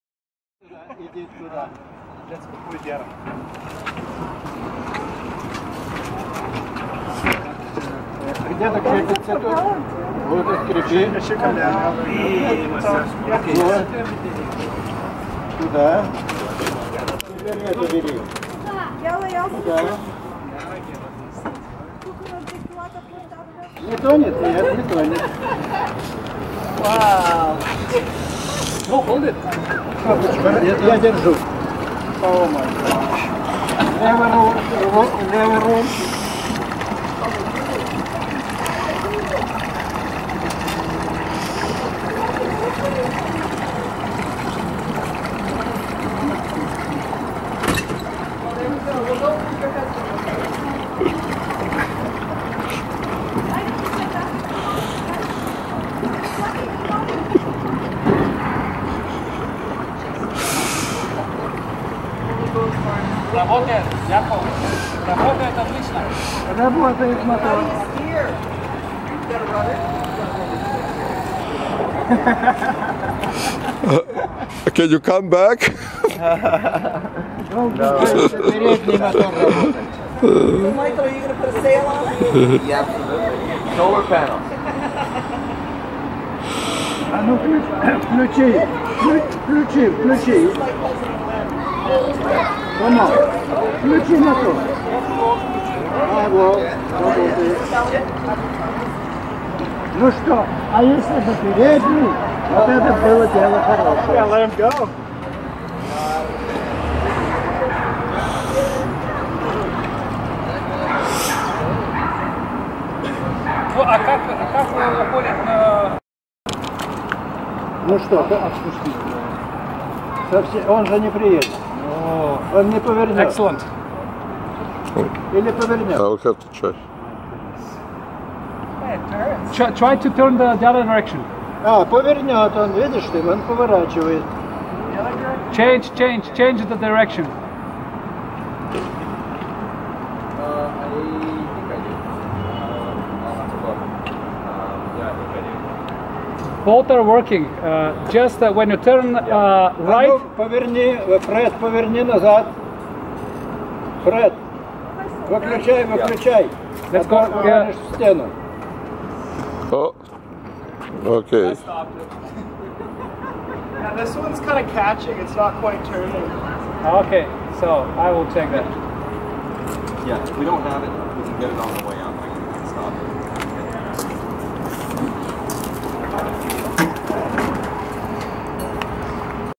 Yakiv's Amphibious Electric Trike - First Test Rides / Sailings
Amphibious Electric Tricycle
Our 'Adopted Uncle' Yakiv never rests with his ideas about alternative transportation. Here is the first test of an Amphibious Electric Tricycle performed yesterday at False Creek Marina. After some initial glitches and mix of all different languages things went smooth sailing ...